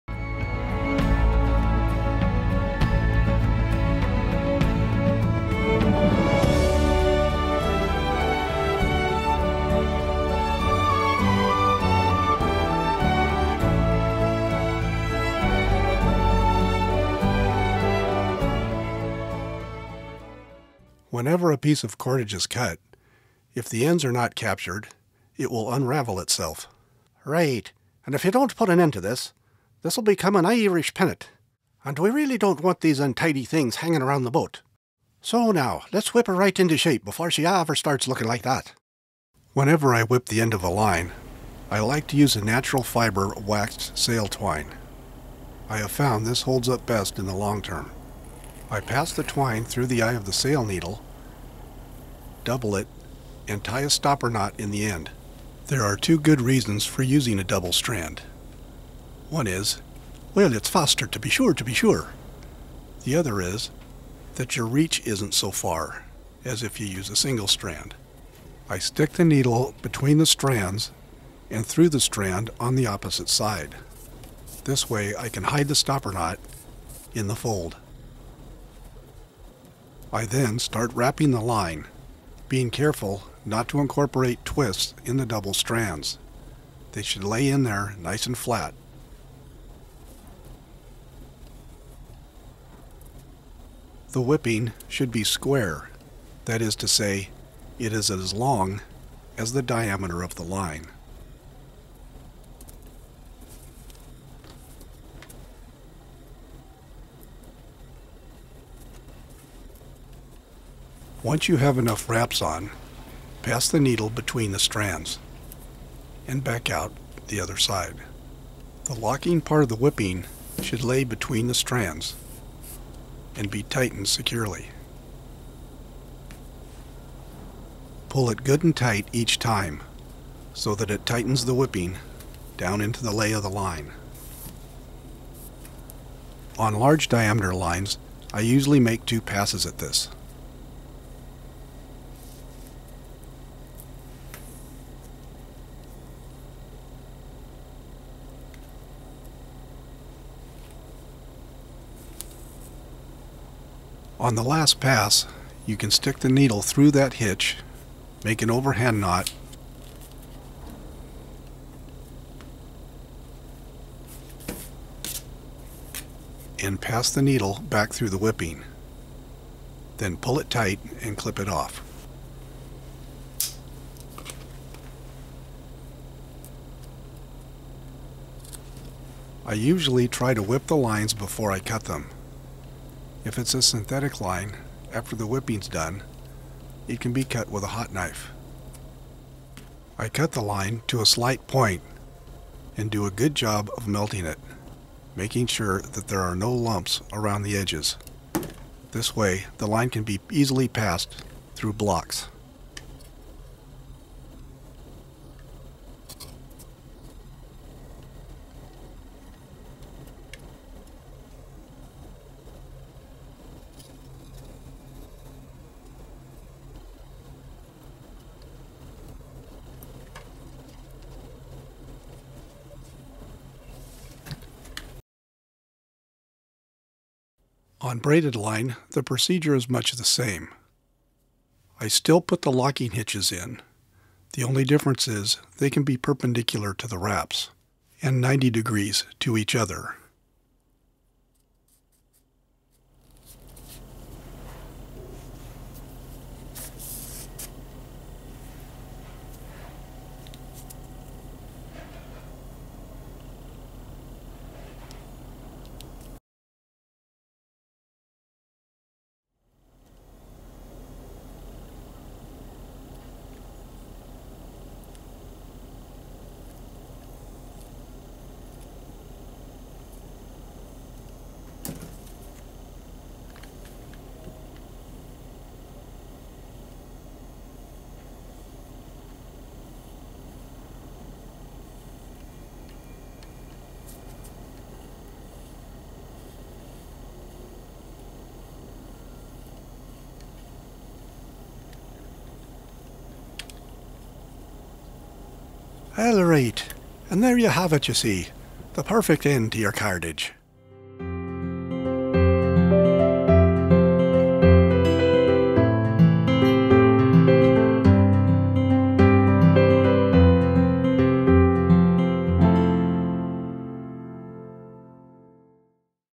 Whipping a line
This video is about Whipping the end of line, stopping the end of a rope from fraying,